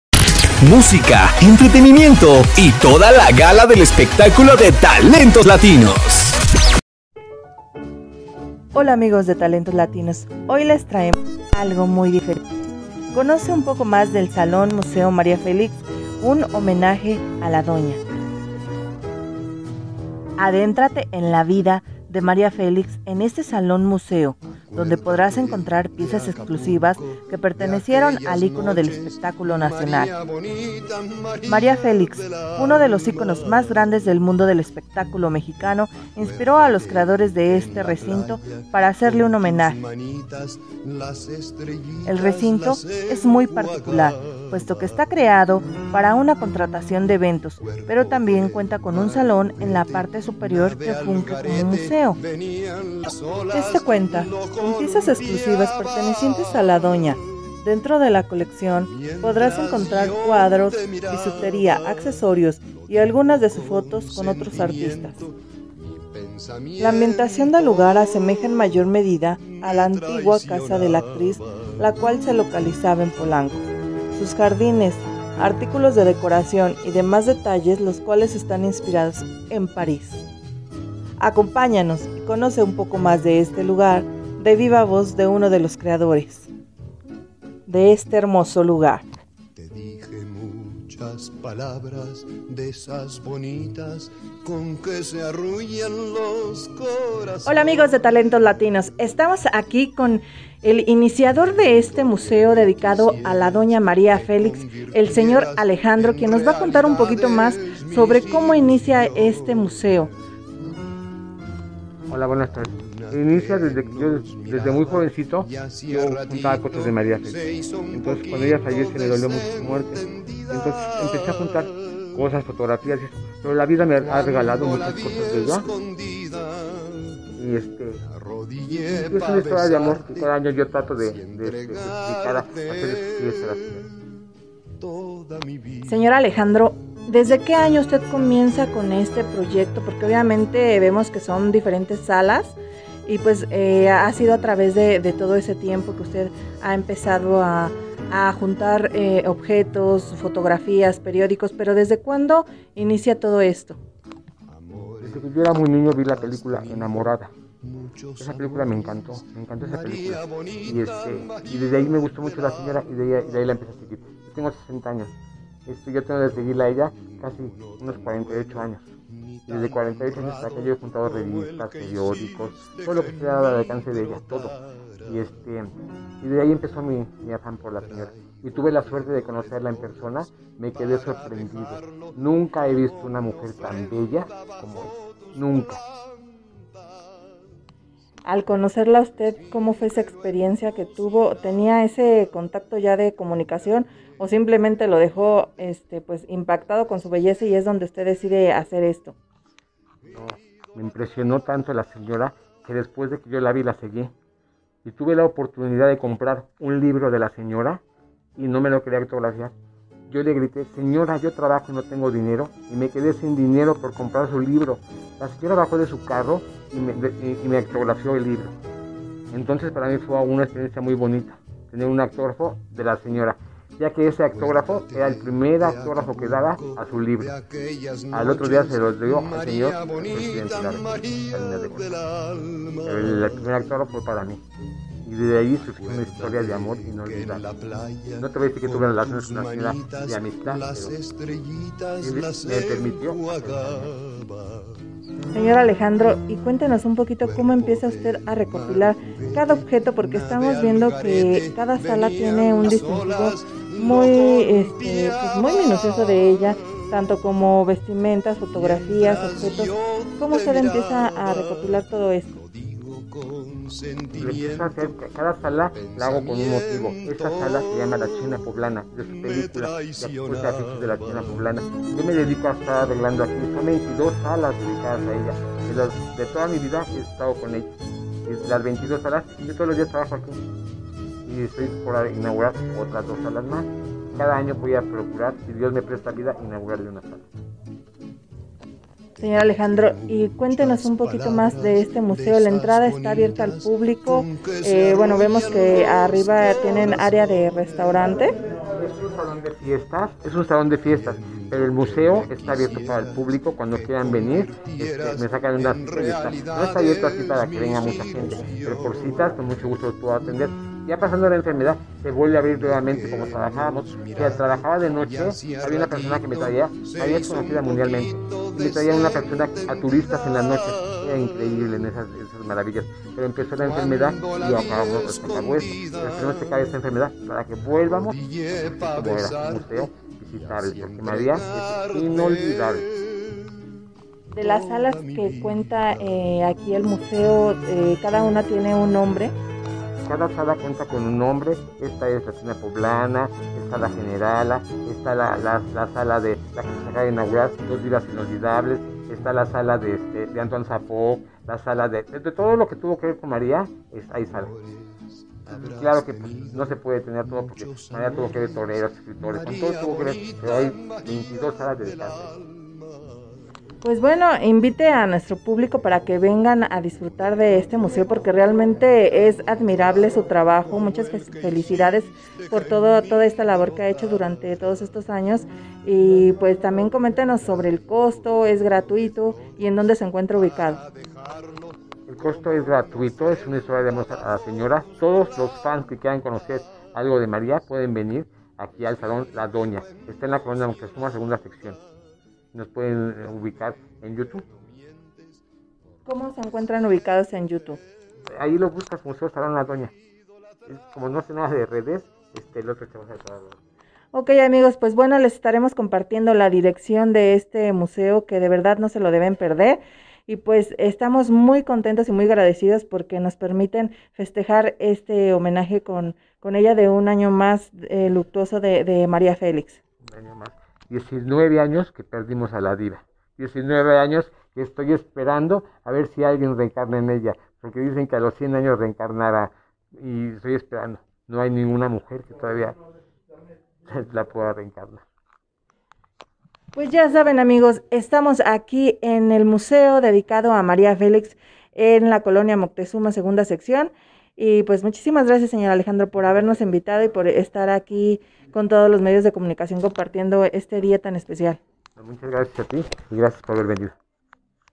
Conoce todo sobre el Salón Museo María Félix y a uno se sus creadores
Salón museo María Félix
Ubicado en Oriente 166 #123 col Moctezuma

Un salón dedicado a la Diva del cine mexicano María Félix donde podrás  disfrutar de objetos originales, fotografías y más.

Creadores del lugar
Alejandro Martínez
Salvador Sánchez